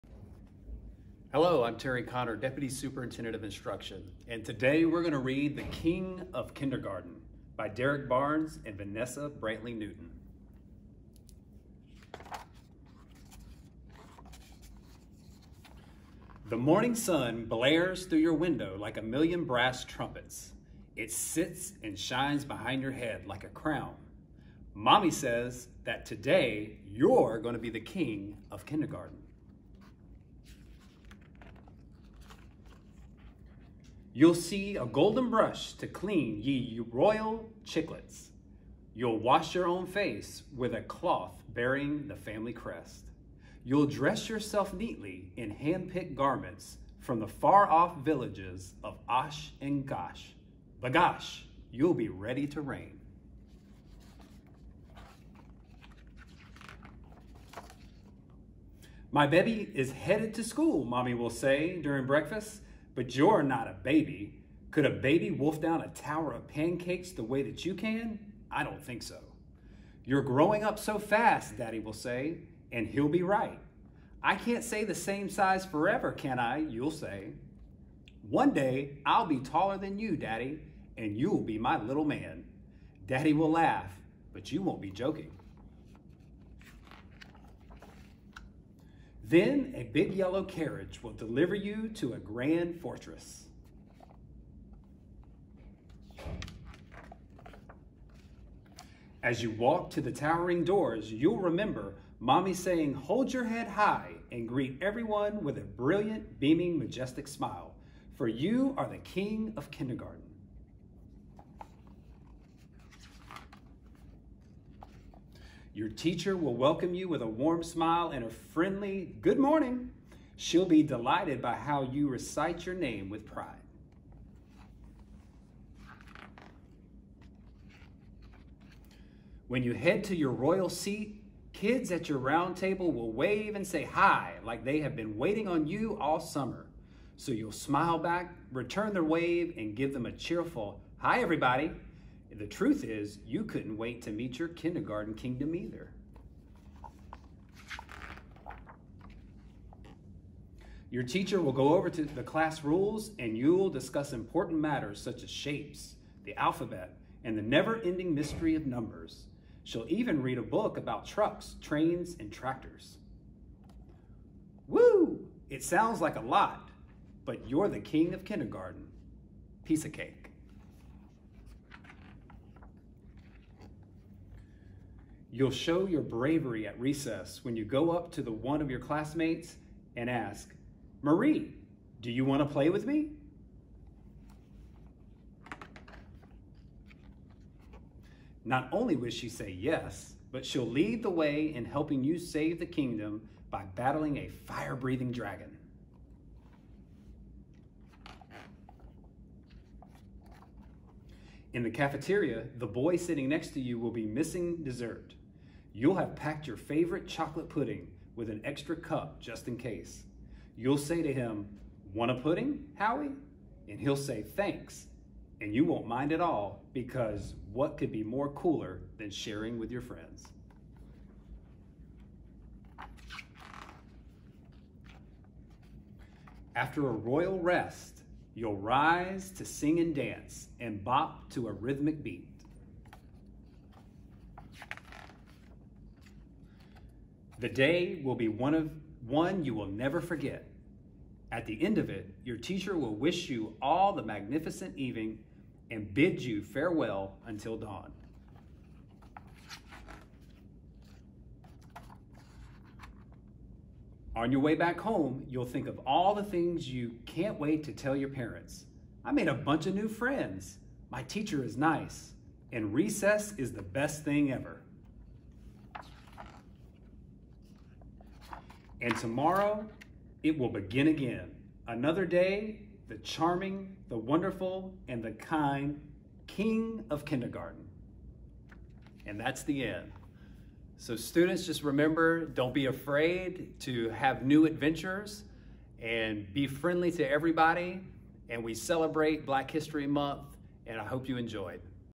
"The King of Kindergarten" by Derrick Barnes and read by Terry Connor (ES)
Terry Connor, Deputy Superintendent of Instruction reads  "The King of Kindergarten" by Derrick Barnes with illustrations by Vanessa Brantley-Newton. This reading is part of our African American Read-in, in honor of Black History Month.